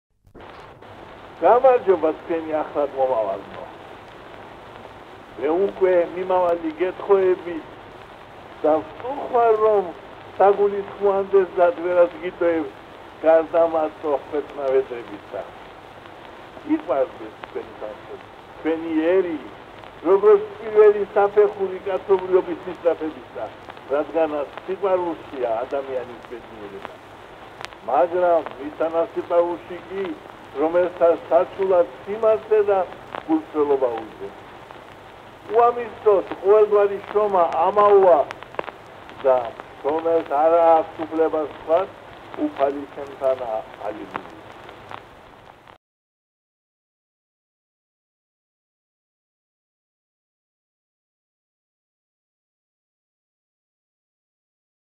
აკაკი წერეთლის „მიმართვა ახალგაზრდობას“. 1913 წელი.
აკაკი წერეთლის 180 წლისადმი მიძღვნილი ეროვნული არქივის ვიდეორგოლი.

კადრები ფილმიდან „აკაკის მოგზაურობა რაჭა-ლეჩხუმში“.
რეჟისორი,ოპერატორი ვასილ ამაშუკელი.
1912 წელი.

აუდიო: აკაკი წერეთლის „მიმართვა ახალგაზრდობას“.
1913 წელი.